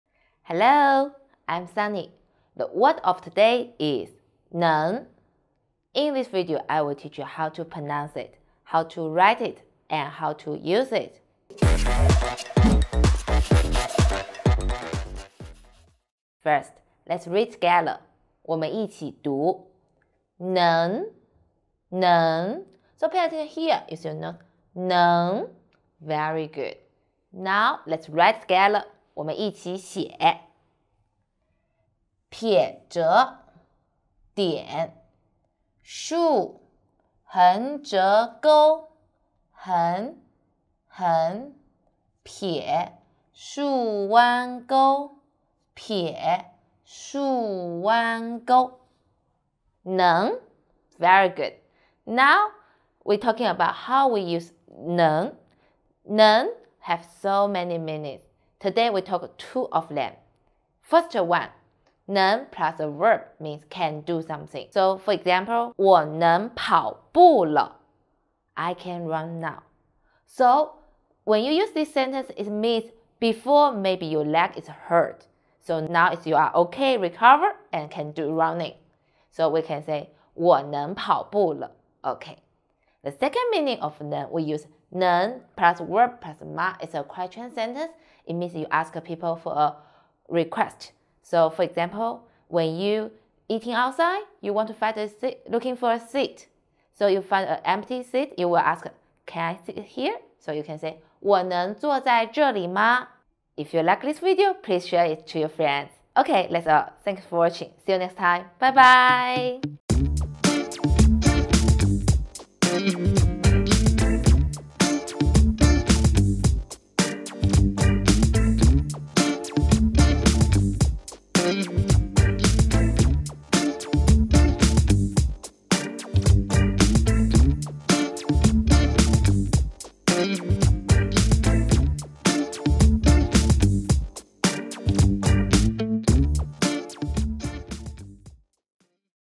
Néng 能 Can Do - Chinese Word of the Day 每日一词
Néng 能 is a word you will be taught in HSK level 1 and is a beginner level word. In this video for néng 能 can do - Chinese Word of the Day 每日一词, you will learn how to pronounce it, how to use it, and how to write the simplified Chinese character.

Watch to the end to learn about stroke order too.
1. 能 means ‘can do’
• Emphasis the innate ability
          Eg. 我有眼睛，我能看。 I have eyes and I can see.

• because of some reasons cannot do something you know how to do before, we can say ‘不能+verb’(no matter the ability is get by learning or not)
          Eg. 我的腿受伤了，不能跑了。
                   My leg is hurt so can not run.
When you recover , then you can say ‘ 能+ verb’  (no matter the ability is get by learning or not)
           Eg. 我现在能游泳了。 Now I can swim.

2. ‘能 +verb +吗 ？’ indicate a request or hope for permission.
   Eg. 我能坐在这儿吗？ Can I sit here？